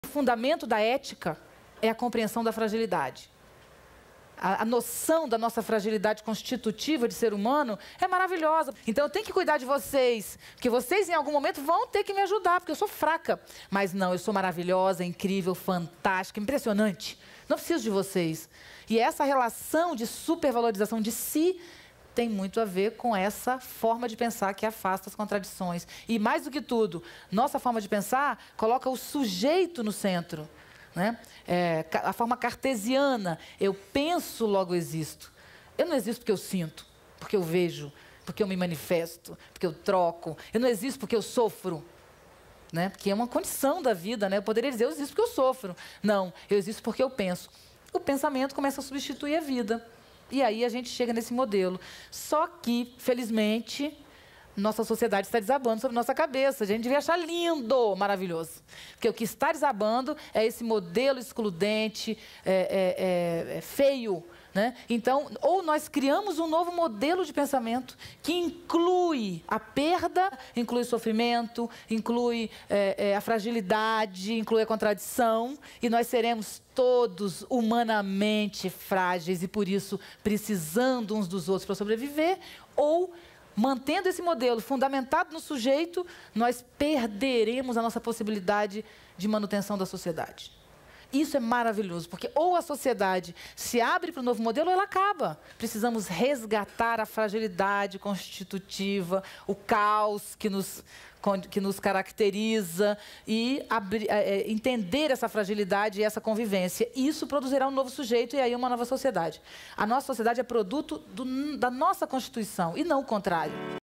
Por que tentamos esconder a nossa fragilidade?
A filósofa Viviane Mosé traz uma reflexão sobre a nossa fragilidade, da qual tentamos não compreender com intuito de nos super valorizarmos como indivíduo. Porém, esse modelo de pensamento está desabando.

Programa exibido em 2008.